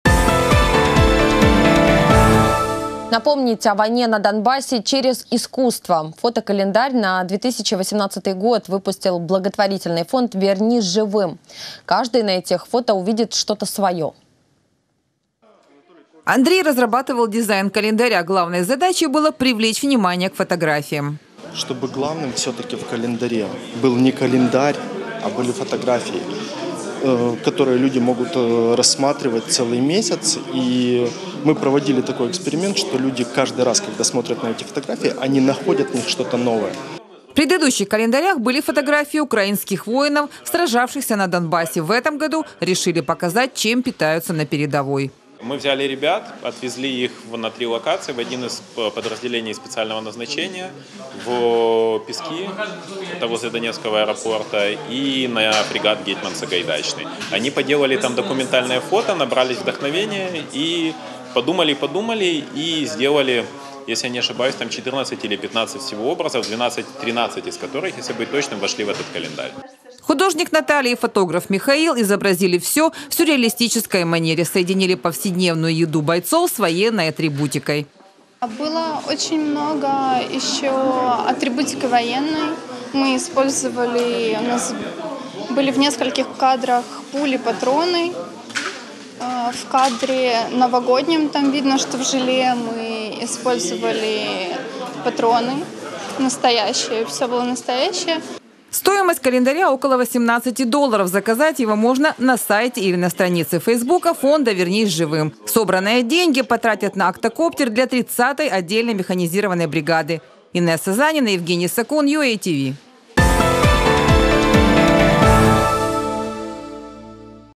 Напомнить о войне на Донбассе через искусство
Фото-календарь на 2018 год  выпустил благотворительный фонд «Вернись живым». Каждый на этих фото увидит что-то свое.